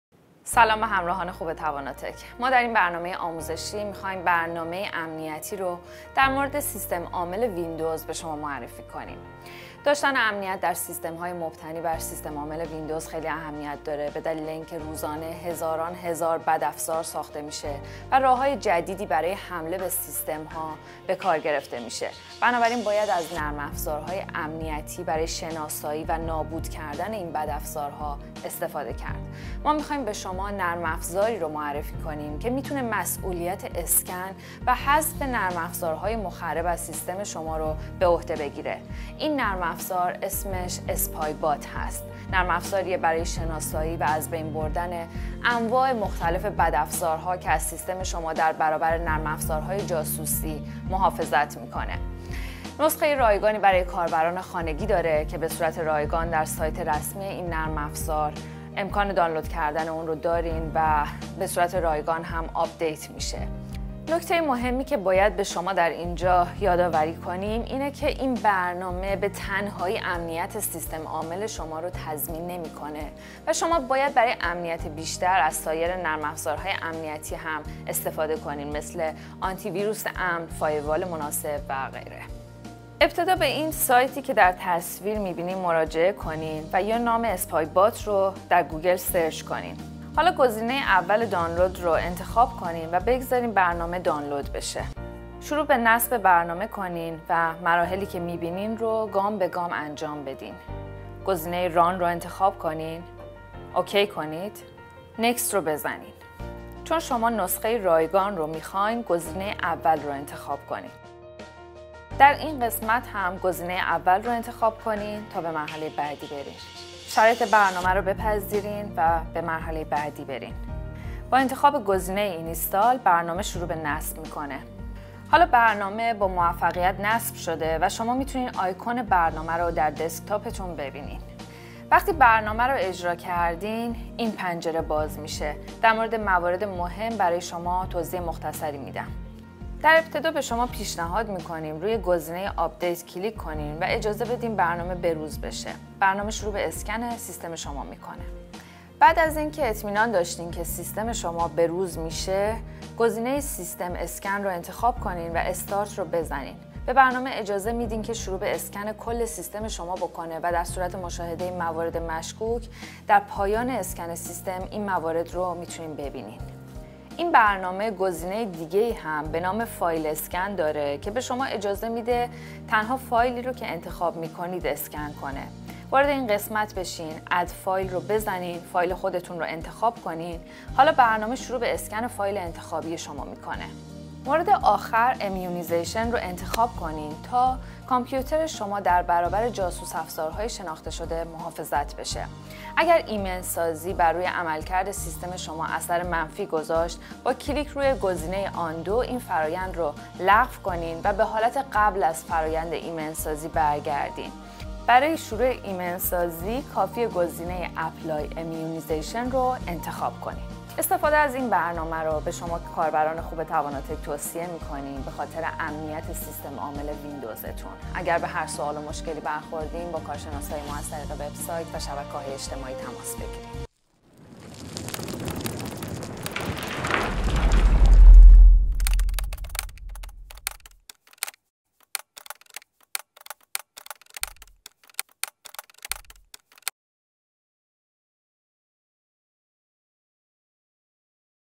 معرفی نرم‌افزار امنیتی Spybot
در این برنامه آموزشی می‌خواهیم برنامه‌ای امنیتی را برای سیستم‌عامل ویندوز معرفی کنیم. داشتن امنیت در سیستم‌های مبتنی بر سیستم عامل ویندوز این روزها بسیار مهم است. روزانه هزاران هزار بدافزار ساخته و راه‌های جدیدی برای حمله به سیستم‌ها به کارگرفته می‌شود.
ما می‌خواهیم به شما نرم‌افزاری را معرفی کنیم  که  اسکن و حذف نرم‌افزارهای مخرب از سیستم شما را انجام دهد.   این نرم‌افزار "Spybot" است.  برای دیدن مطالب بیشتر به وب‌سایت تواناتک بروید: